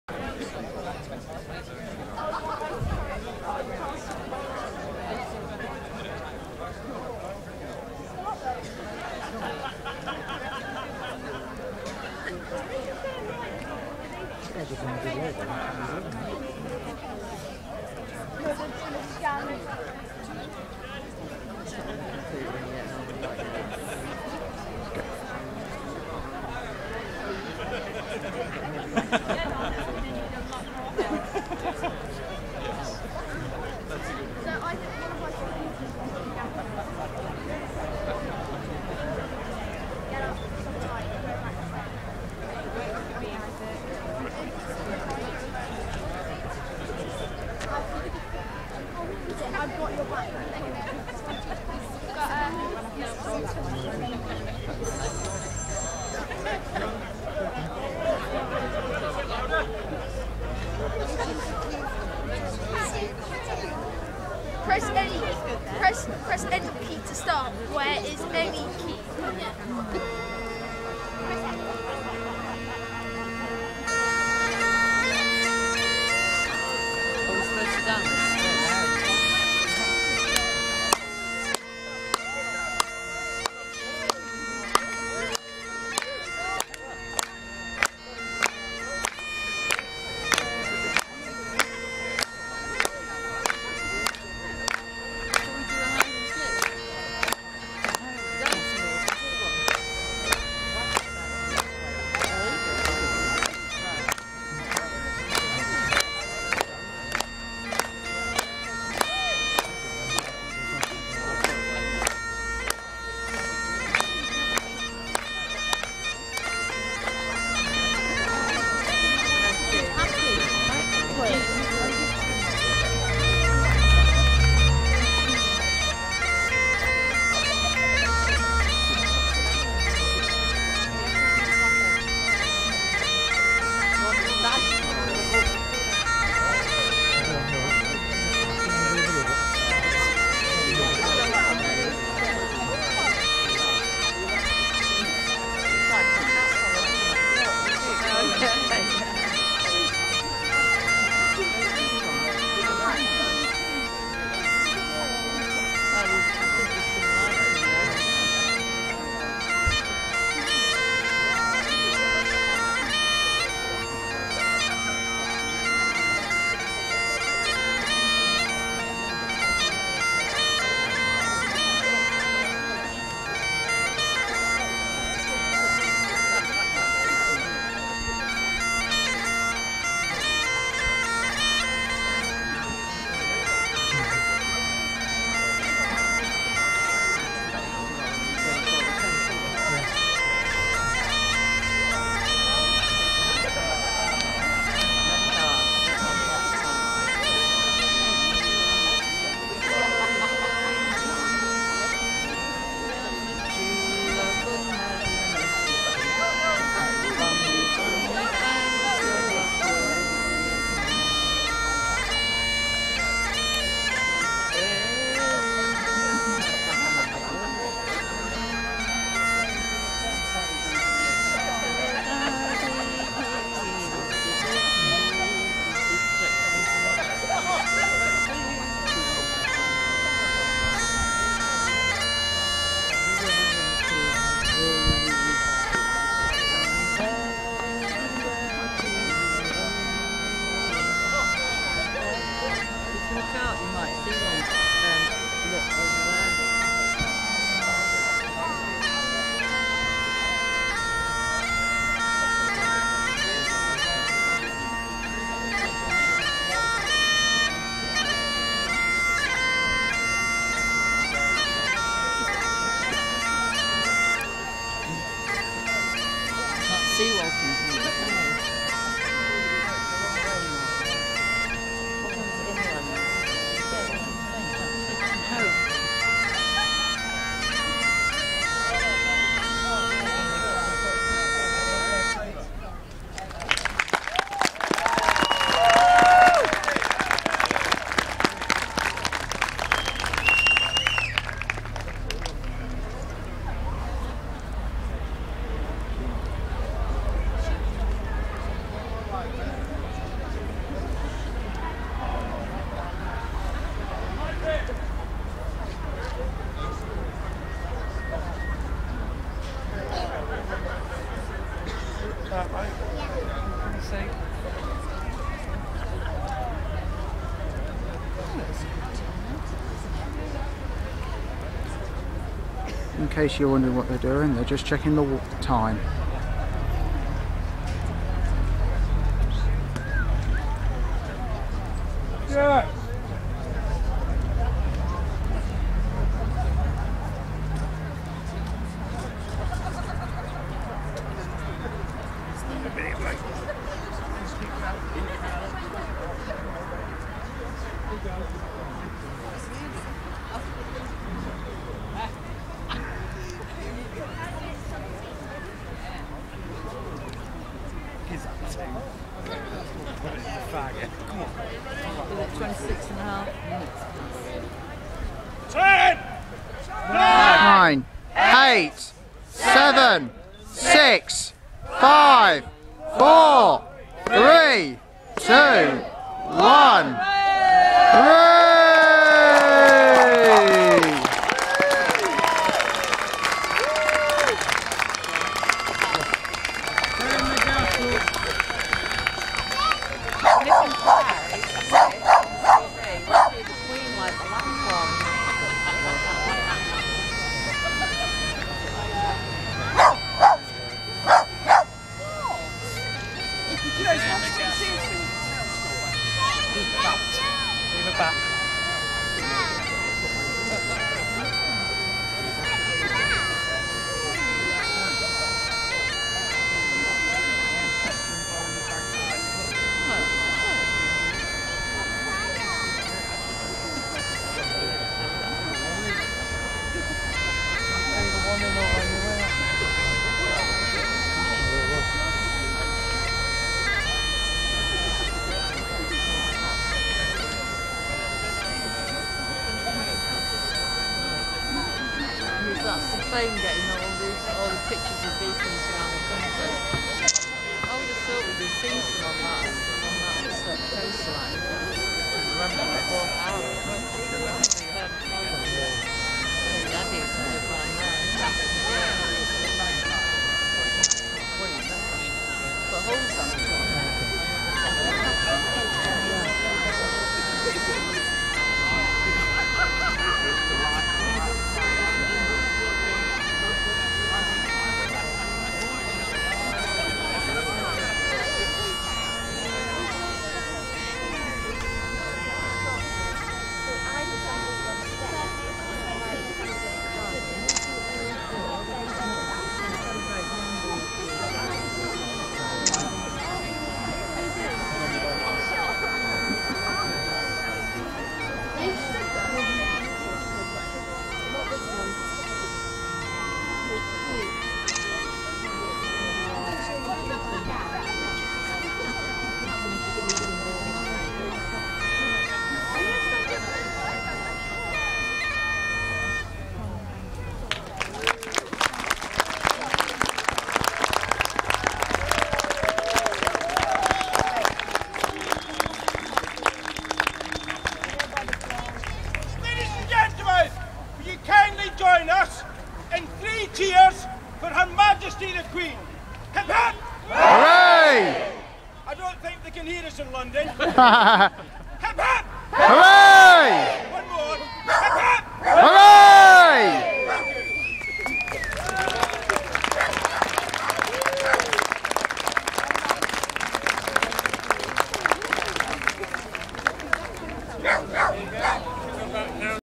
Ross Castle Beacon, Cleethorpes Diamond Jubilee
The lighting of the Beacon at Ross Castle in honour of HM Queen Elizabeth II on sixty years on the throne on the night of Monday 4th June 2012